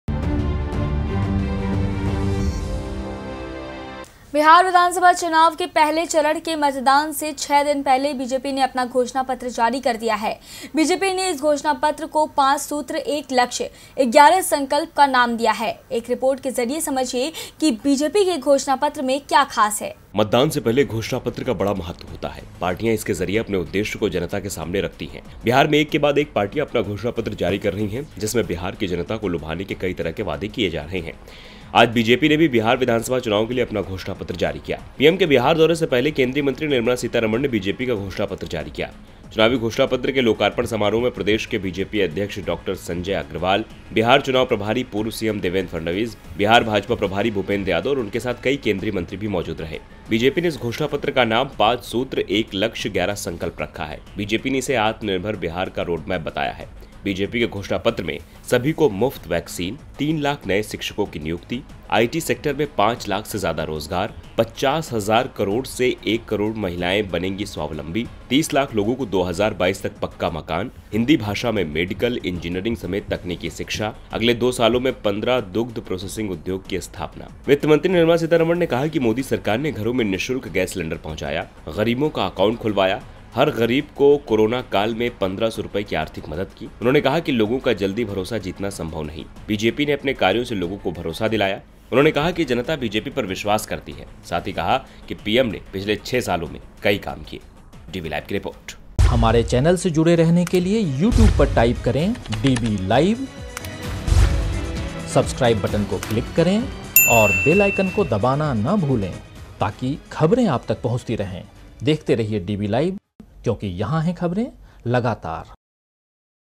Bihar Elections के लिए BJP ने जारी किया Manifesto
BJP का घोषणा पत्र जारी | 5 सूत्र, एक लक्ष्य, 11 संकल्प रखा घोषणा पत्र का नाम